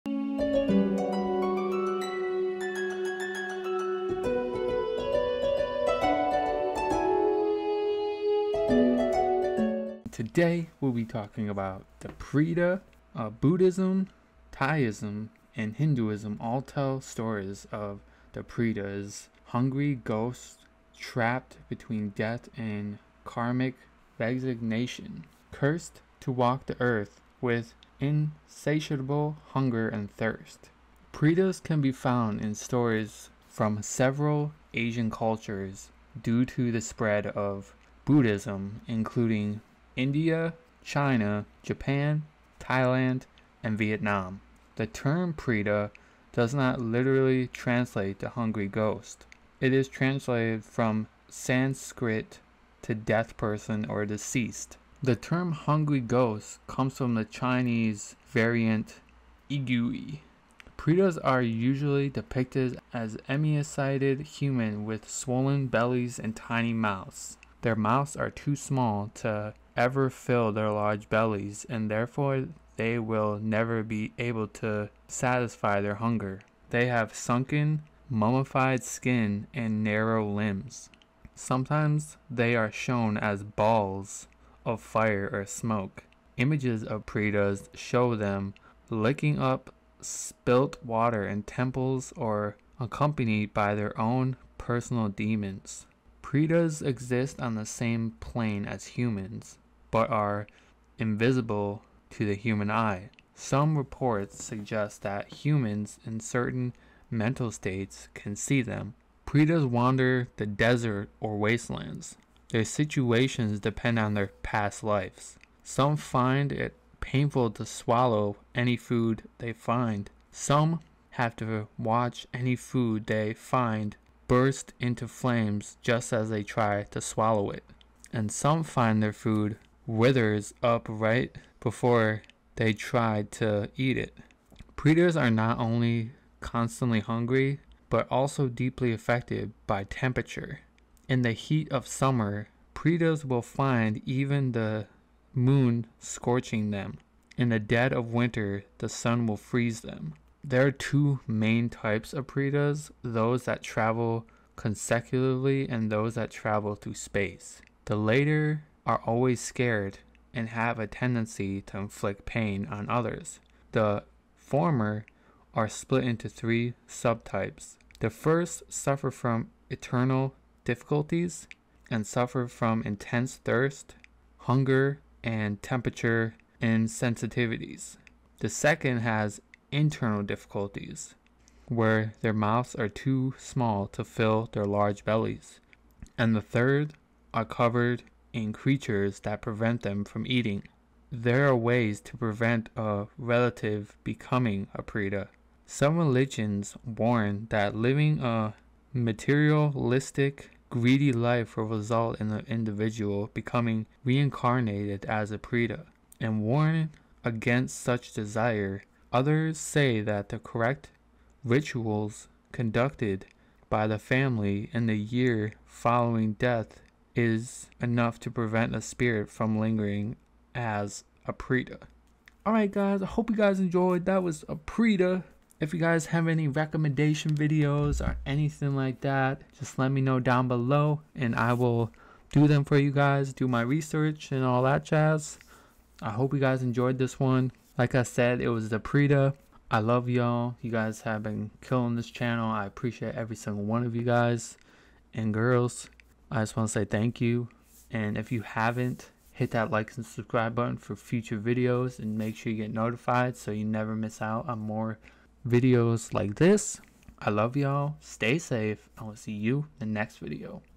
What are Preta
In this video, I will be talking about what are preta's. I hope you all enjoy; I love you all. Thank you for the support. Stay Safe.